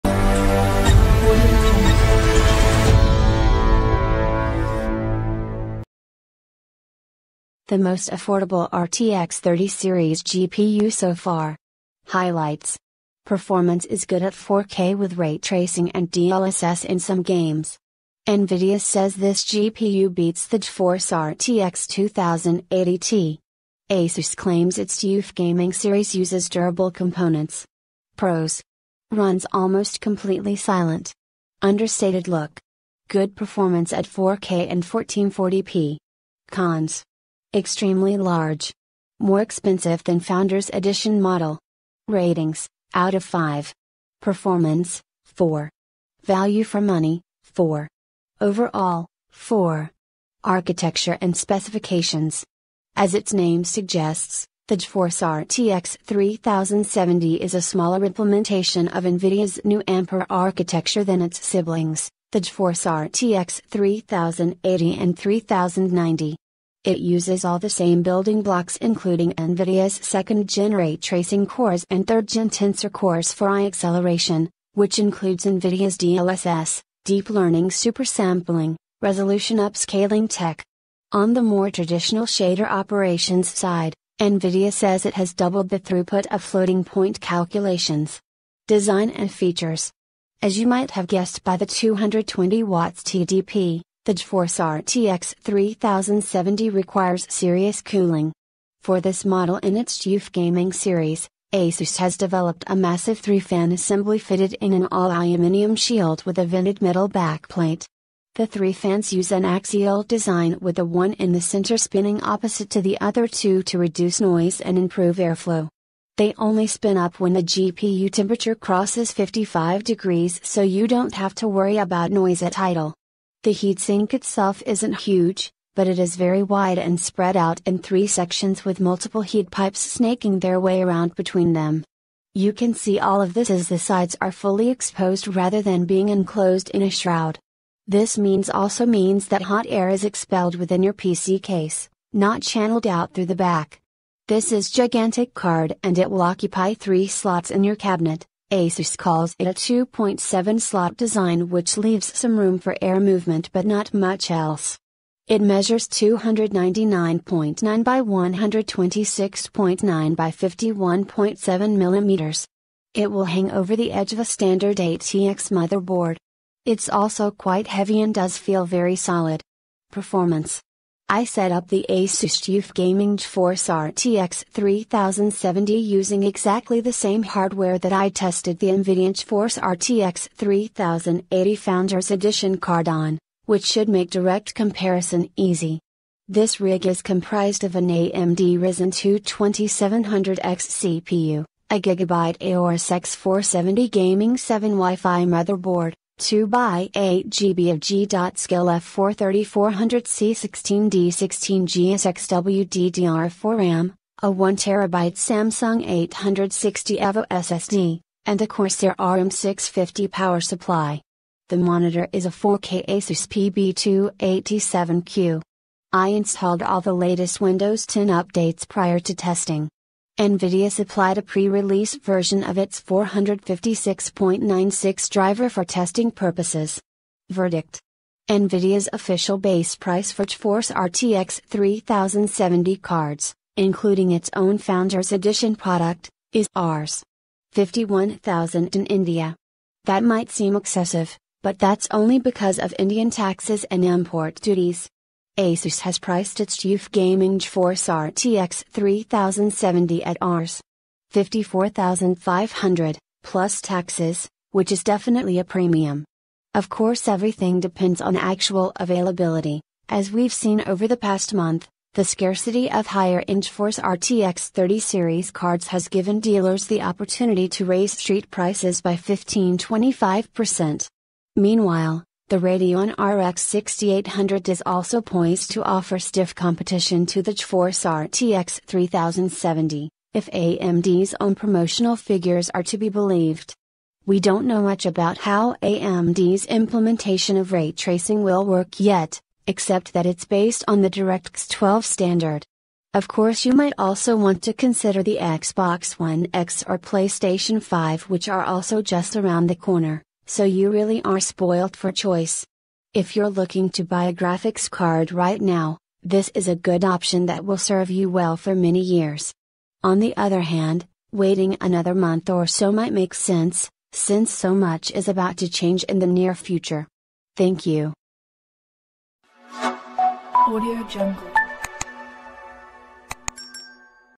Asus TUF Gaming GeForce RTX 3070 Review
srmqf924ug.blogspot.com The most affordable RTX 30-series GPU so far.

HIGHLIGHTS

Performance is good at 4K with ray tracing and DLSS in some games
Nvidia says this GPU beats the GeForce RTX 2080 Ti
Asus claims its TUF Gaming series uses durable components

Pros

Runs almost completely silent
Understated look
Good performance at 4K and 1440p
Cons

Extremely large
More expensive than Founders Edition model
Ratings (out of 5)

Performance: 4
Value for Money: 4
Overall: 4

Nvidia's GeForce RTX 30 series has proven to be a bit controversial thanks to extremely low availability and inflated prices. Despite a slick launch presentation and largely favourable reviews for the high-end GeForce RTX 3080 and ultra-premium GeForce RTX 3090 GPUs, buyers have been frustrated and upset. Many are now saying they'll hold off till reviews of AMD's just-announced Radeon RX6000 series are out. No matter how good the products are, they're useless if buyers can't get their hands on them. Now, it's time for the more mainstream GeForce RTX 3070 to hit the market, and demand is naturally expected to be even higher.

The GeForce RTX 3070 GPU is still a high-end piece of hardware aimed at enthusiasts, and graphics cards based on it are not going to be considered affordable, especially in India. Nvidia continues to position its RTX series with ray tracing as premium offerings. The RTX 3070 will cater mainly to those who want to play today's games at the highest settings at 1440p, or who don't mind a few tradeoffs at 4K.

Nvidia GeForce RTX 3070 architecture and specifications

As its name suggests, the GeForce RTX 3070 is a smaller implementation of Nvidia's new Ampere architecture than its siblings, the GeForce RTX 3080 and 3090. It uses all the same building blocks including Nvidia's second-gen ray tracing cores and third-gen tensor cores for AI acceleration, which includes Nvidia's DLSS (Deep Learning Super Sampling) resolution upscaling tech. On the more traditional shader operations side, Nvidia says it has doubled the throughput of floating-point calculations.

DLSS is something that becomes more important with non-flagship GPUs since its whole purpose is to compensate for low frame rates caused by the massive demand that ray tracing can put on your hardware. DLSS essentially renders a game at a lower resolution, which is less resource-intensive, and then upscales the output to your desired target. When first introduced with the RTX 20-series (Turing architecture), it didn't have much traction, but has since been adopted by more and more game developers. Nvidia even says that DLSS can sometimes deliver sharper graphics than native rendering.

Ray tracing allows for incredibly realistic spot and area lighting, shadows, reflections and refractions, and more. This of course means that graphics are much more immersive, and in the future we might see gameplay mechanics that actually depend on ray tracing. However, the performance penalty is enormous so the benefits are out of reach for many gamers. If Ampere's generational improvements can bring better performance to lower prices, ray tracing will have a much better chance of going mainstream.

Speaking of generational improvements, Nvidia claims that the GeForce RTX 3070 delivers performance at 1440p that outshines the previous top-end GeForce RTX 2080 Ti for less than half the cost. It is also said to be as much as 60 percent faster than the GeForce RTX 2070, its direct predecessor.

And it isn't just gameplay – Nvidia says its Ampere architecture brings new benefits and better performance to related activities such as streaming or broadcasting, as well as using game engines to create realistic animation. Tensor cores, for example, can be used to help improve voice and video quality as well as cancel noise and replace your background while livestreaming.

In terms of numbers, the GeForce RTX 3070 GPU is equipped with 5,888 CUDA cores, 184 tensor cores and 46 RT cores, organised into 46 clusters called Streaming Multiprocessors. The GPU runs at up to 1725MHz. All GeForce RTX 3070 graphics cards will have 8GB of GDDR6 RAM on a 256-bit bus for a total memory bandwidth of 448GBps. The official TDP for Nvidia's Founders Edition is 220W. Nvidia's partners including Asus are free to tweak speeds and some other hardware, most notably the designs of the PCB and cooling apparatus.

The HDMI 2.1 standard allows for 4K 120Hz over a single cable, and you also get DisplayPort 1.4a outputs. Another forward-looking feature is support for Microsoft's upcoming DirectStorage API which will allow games to load data directly from storage devices to a GPU's memory pool rather than going through system memory first. This could make waiting for levels to load a thing of the past, and frees the CPU from having to decompress huge textures.